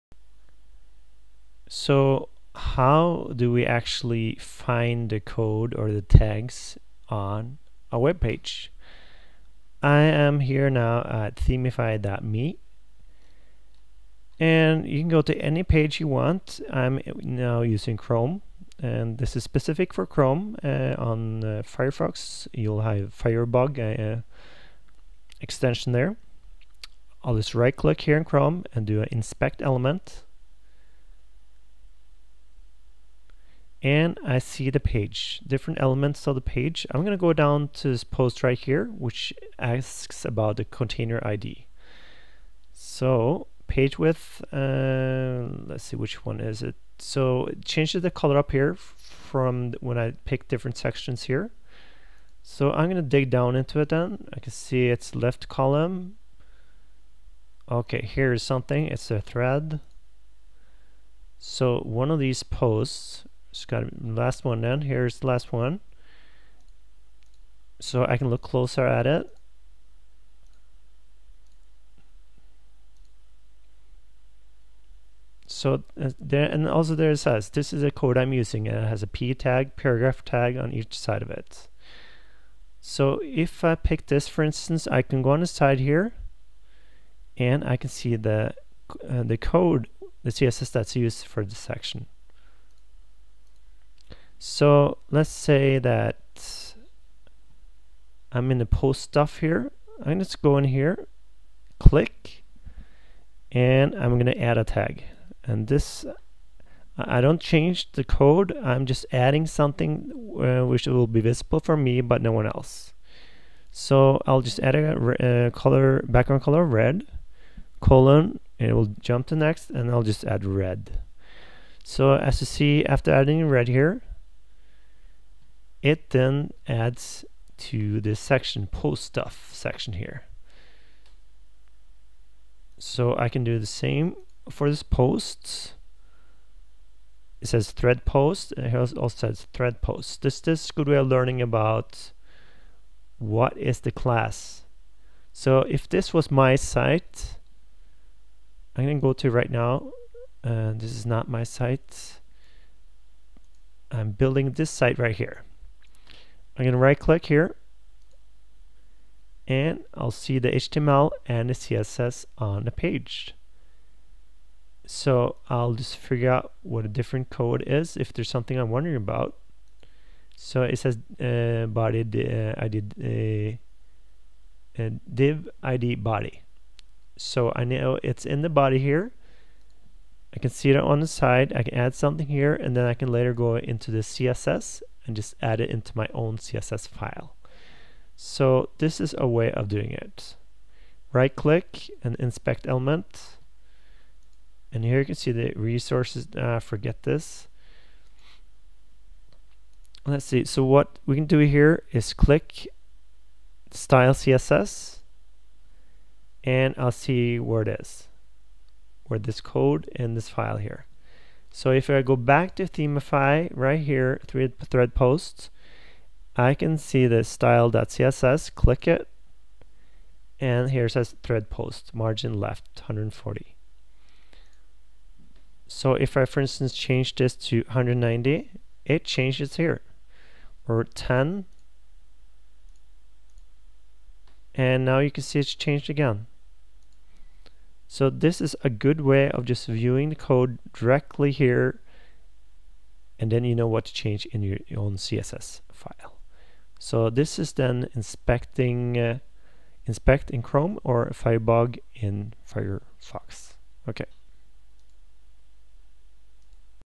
How to inspect - view html and css code
This is a good way of learning about different elements on a web page. Through Chrome right click and selecting Inspect or Firebug through Firefox.
So when you do not know the tag then you can use the above and see which tag is used and use the same tag in your own CSS file. Just select the code and copy it and paste it into your CSS style file. The video is not as clear as my other videos but I will add it anyway.